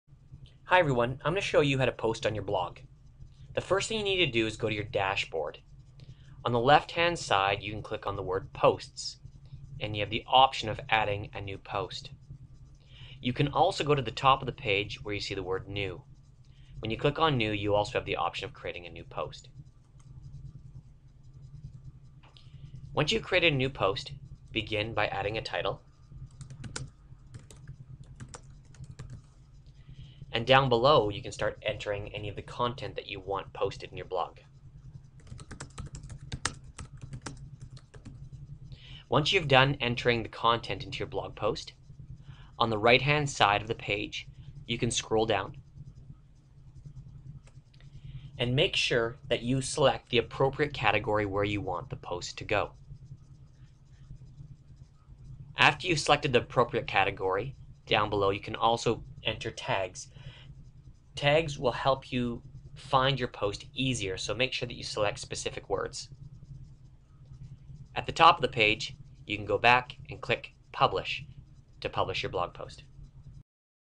Edublogs: How To Post on Your Blog
Learn how to create post to your Edublog.